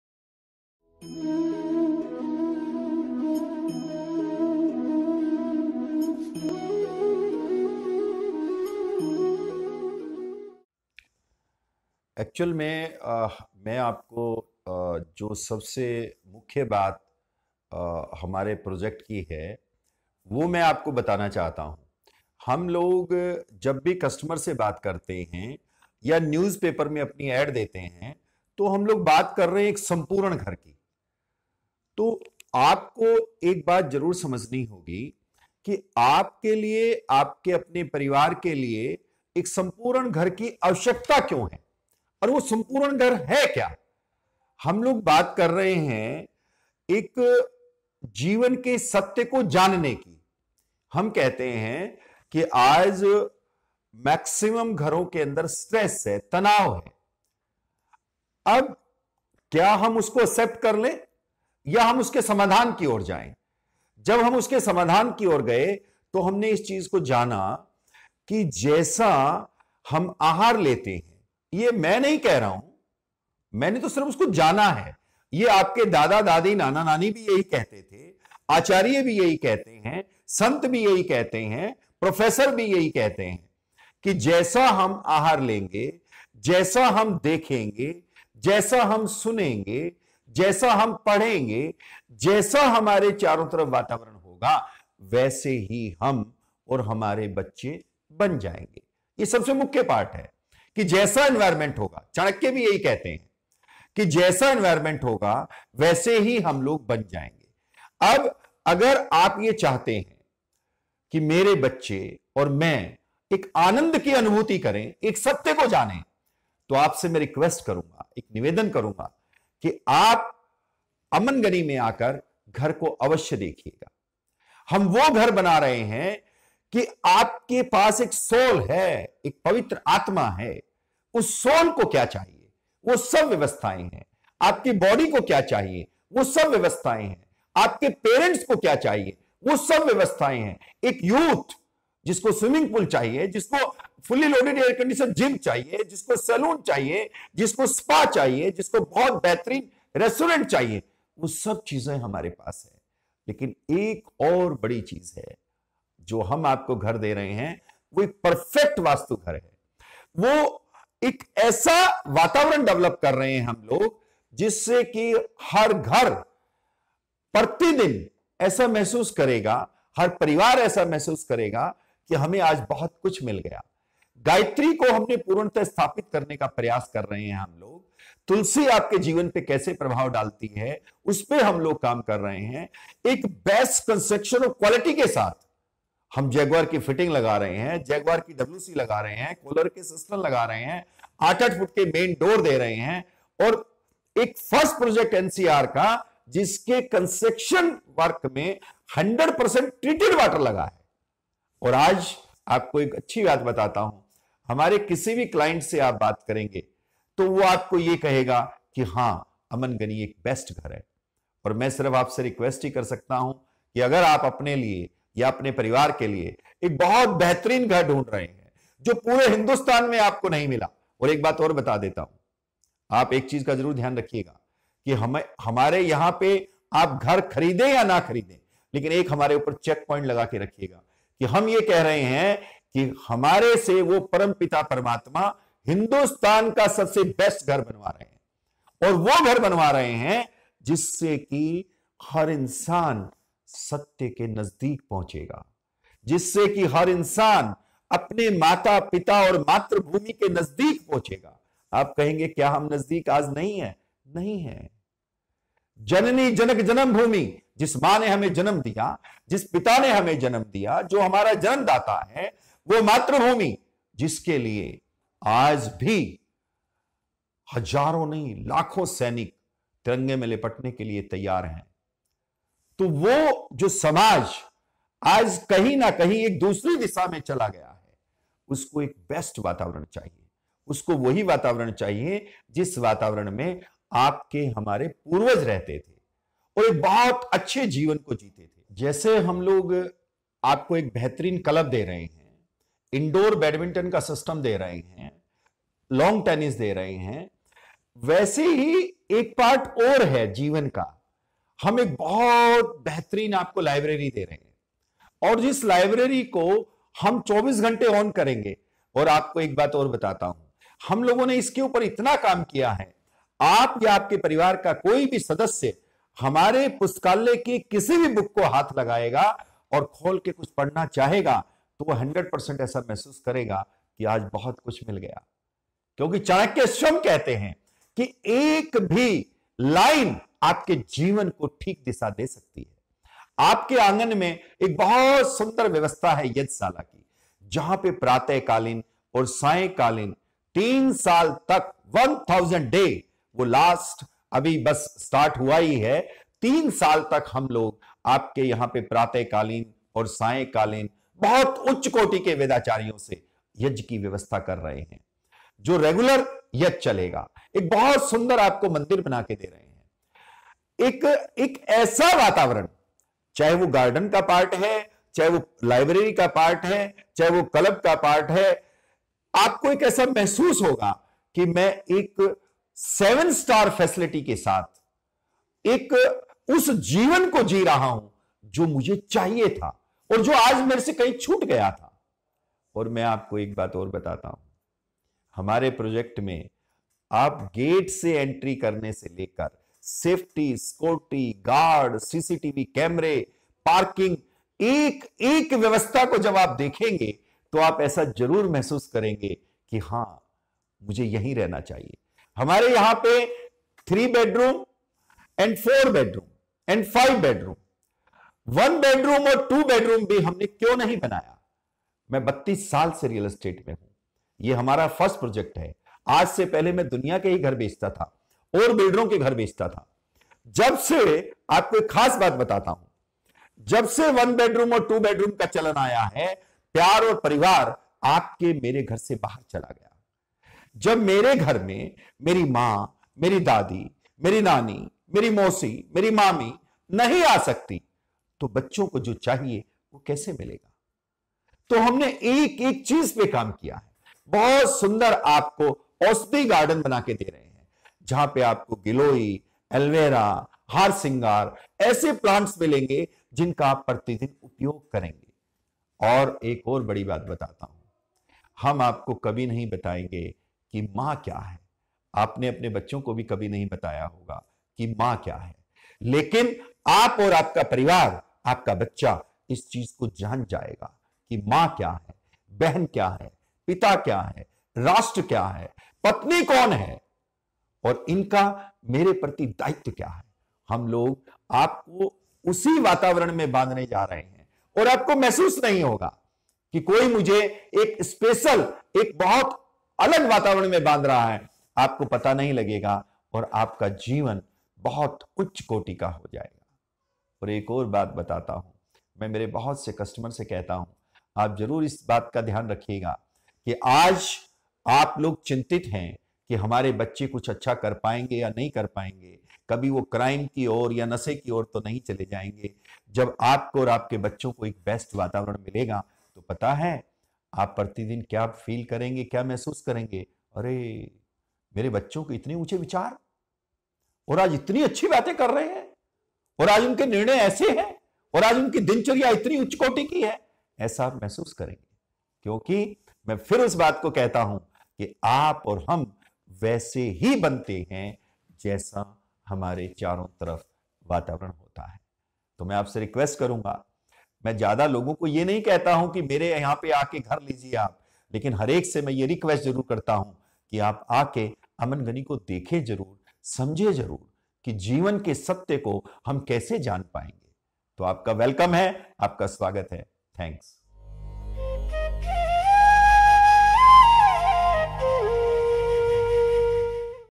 Amangani Rewari | Amangani Peaecful Homes Sector 25 Rewari @propertychahiye 📞
Amangani Peaecful Homes Sector 25 Rewari Presenting a premium set of 3BHK & 4BHK Apartments at Sector 25, Rewari, in the heart of the city.At Amangani, we just dont offer you home made of bricks, here we welcome you to a place that reaffirms your faith in the beautiful sight of life. Where you learn the true beauty of human values and live in peace in every moment.